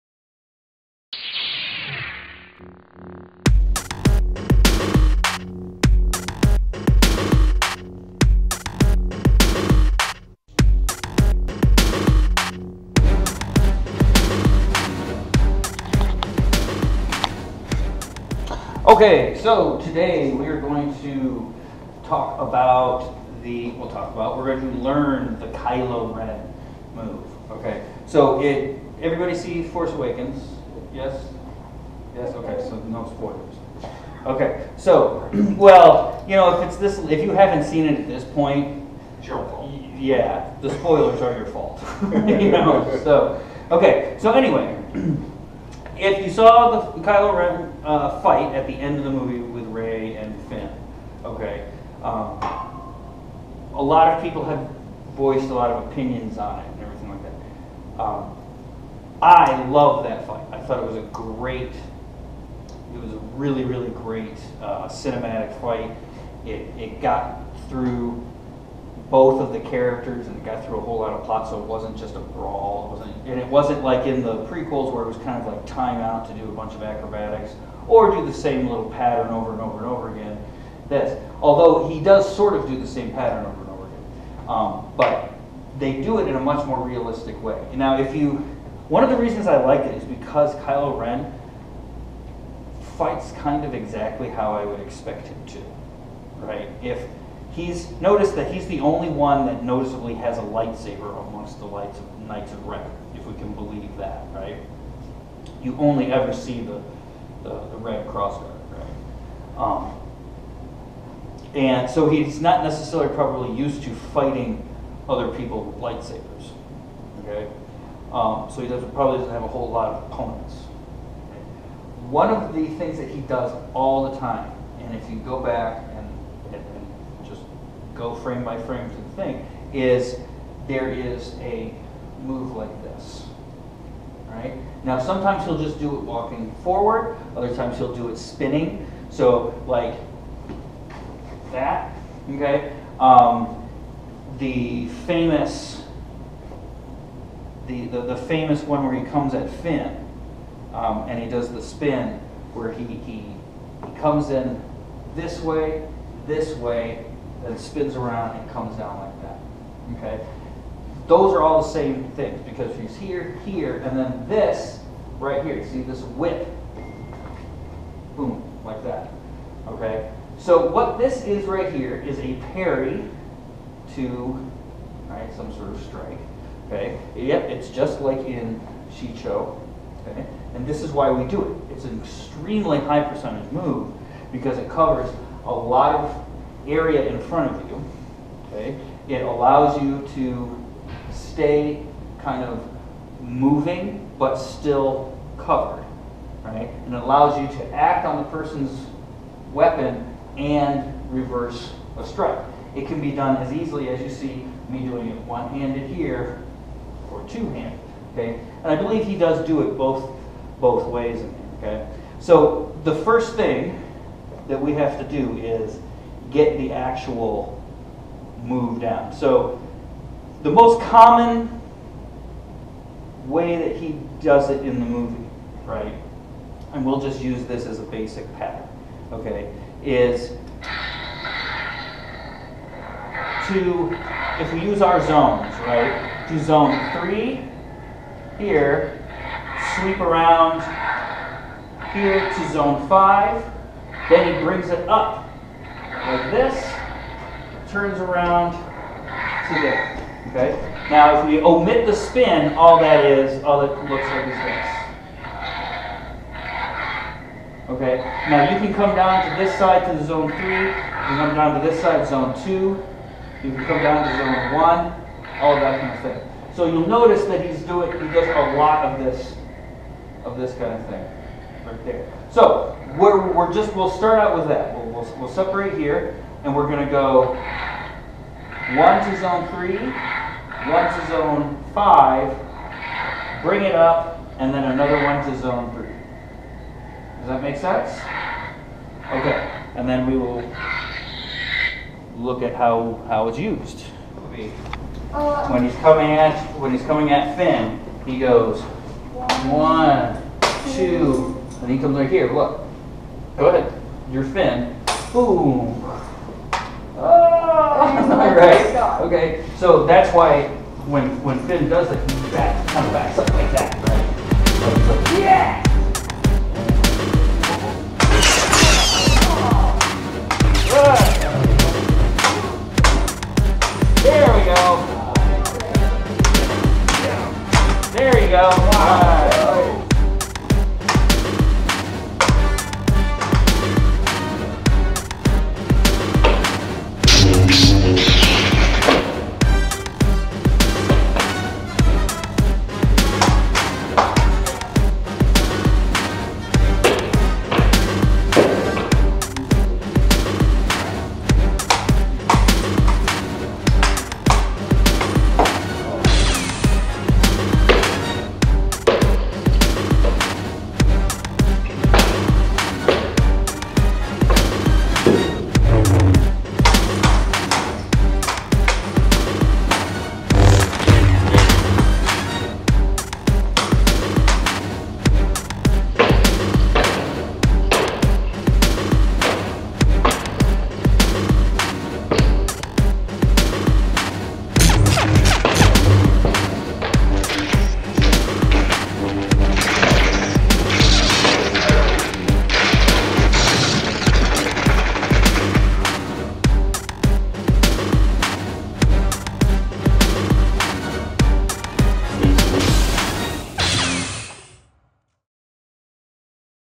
Saberfit: Fighting like Kylo
We discuss the main technique Kylo Ren uses in The Force Awakens!